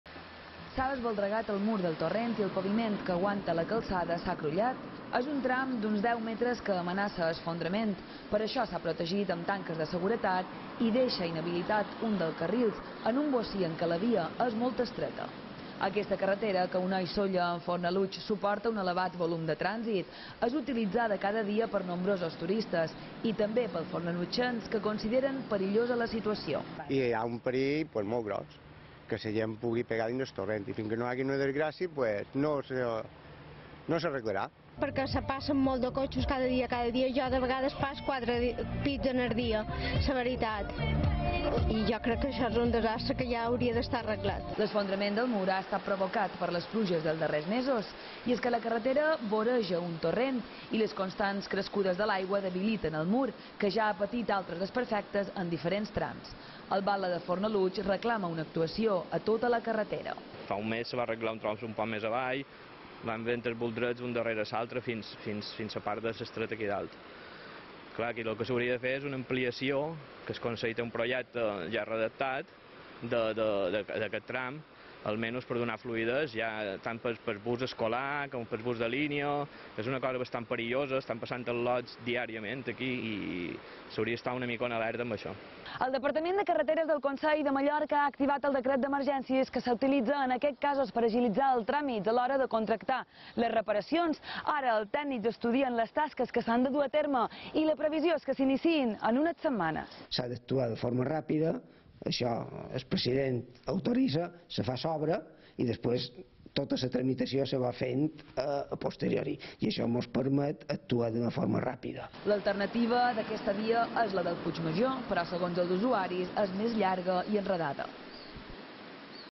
Malestar entre les veïnats de Fornalutx per l’esfondrament de la carretera que va a Sóller
L’esfondrament d’un bocí de mur de la carretera que uneix Sóller amb Fornalutx genera malestar entre els usuaris que hi passen diàriament. El crull que ha aparegut damunt l’asfalt ha deixat inhabilitat un carril en un tram molt estret, i consideren que la situació és perillosa. És una carretera que passa per devora un torrent i que ja ha patit aquest tipus de desperfectes en altres ocasions.